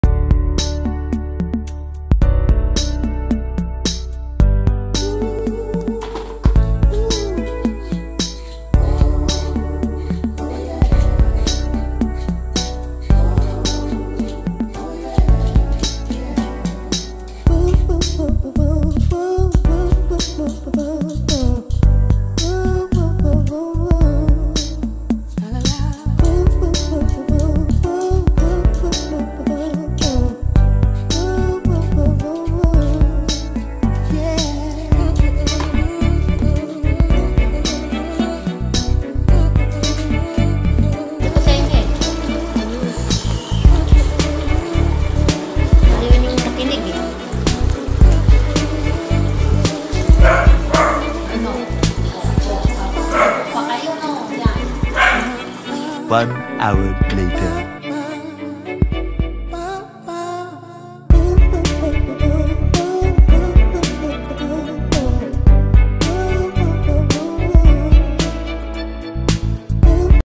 YUMMY AND TASTY CHICKEN SARCIADO
Featuring recipe: "Chicken Sarciado"

In celebration of death anniversary of her mom.